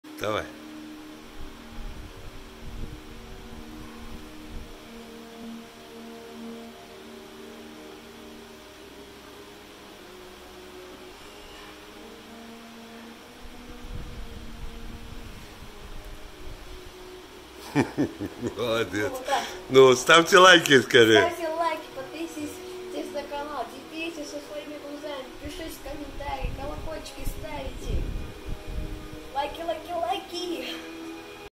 nine-year-old boy bends steel tent pegs / Девятилетний мальчик гнёт стальные колышки от палатки
A nine-year-old boy bends steel tent pegs after the first week of training with the "well" arm roller exerciser

Девятилетний мальчик гнёт стальные колышки от палатки после первой недели тренировки с тренажёром "колодец"